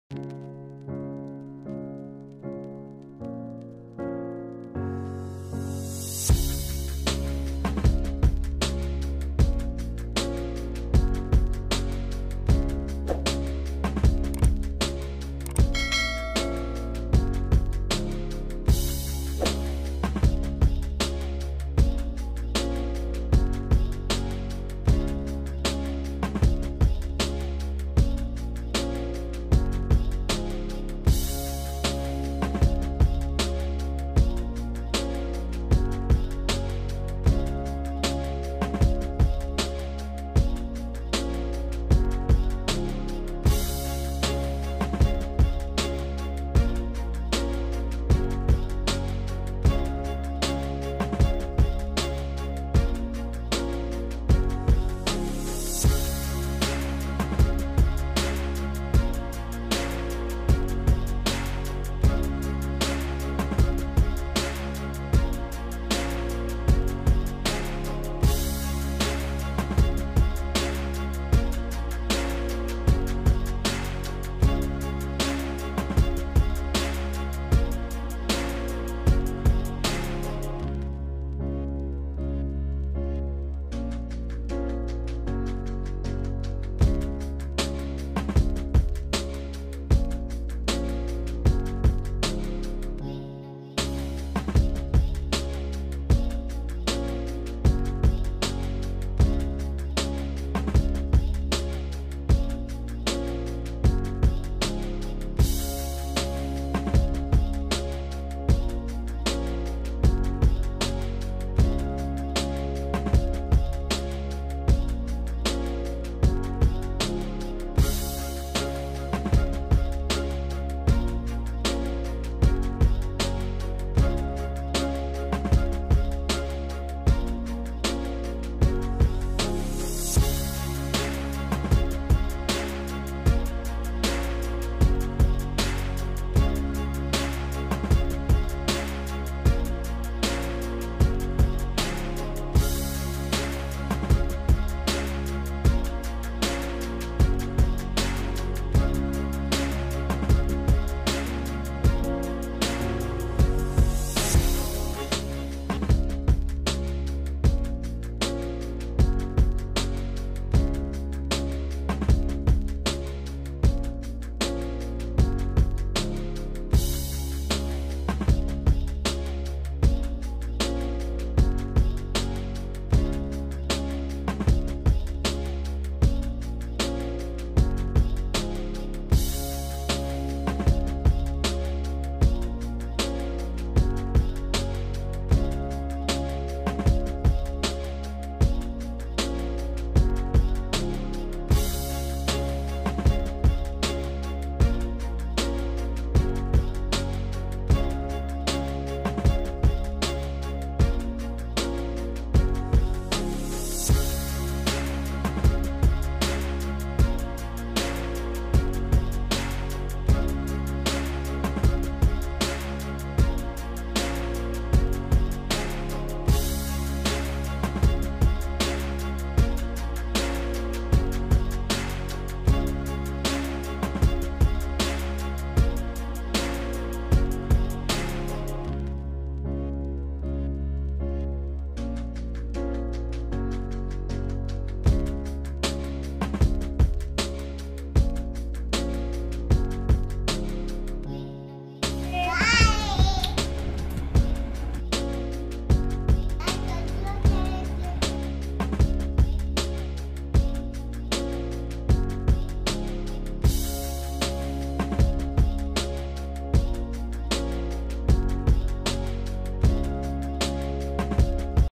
How to make a coffee mug with blocks.
Learn how to make a coffee mug with blocks.

Blocks building, blocks game, blocks fun
Blocks creativity, blocks activity.

Let's play &enjoy with HusainHabeeb.

Like I share &comments &